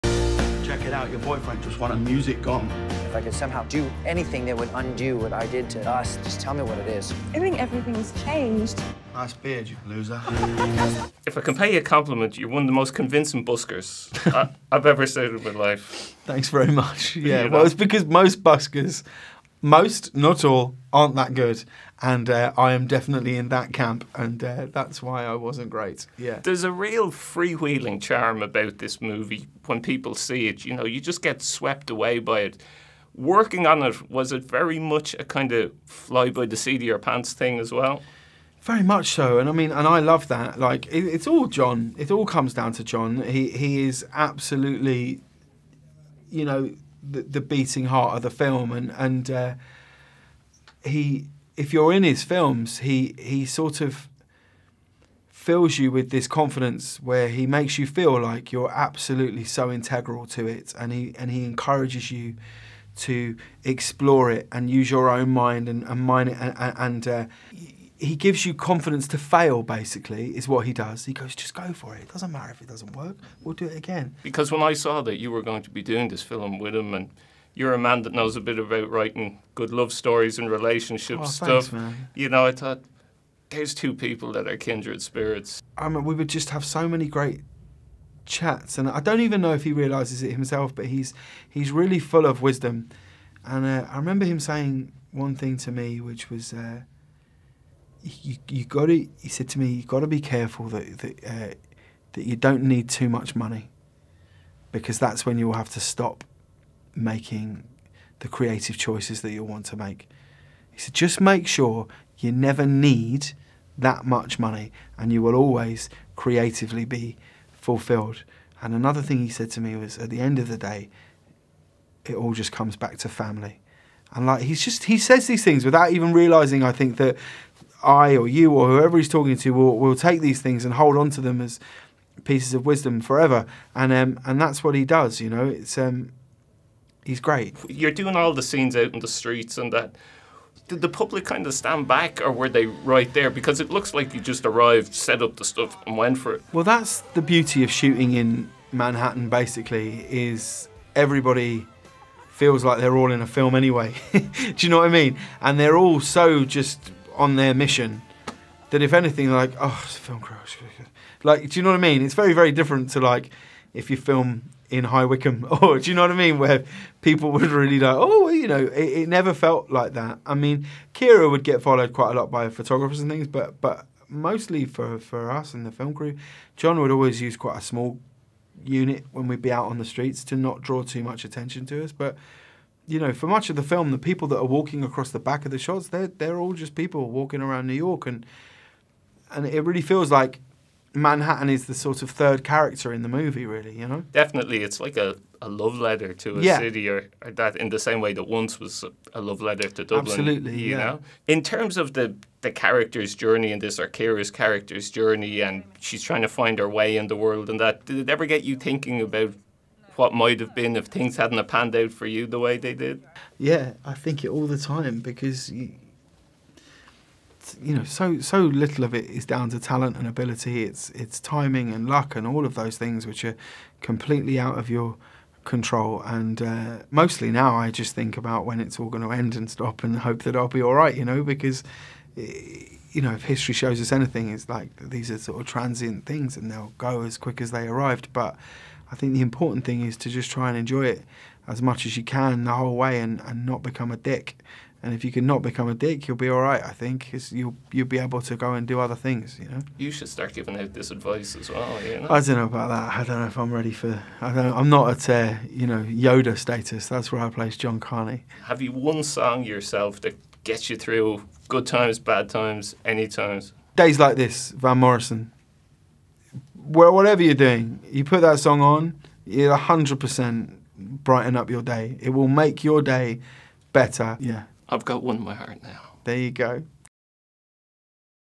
James Corden on Begin Again
James Corden tells RTÉ TEN's Harry Guerin about working with Irish writer-director John Carney on his film Begin Again.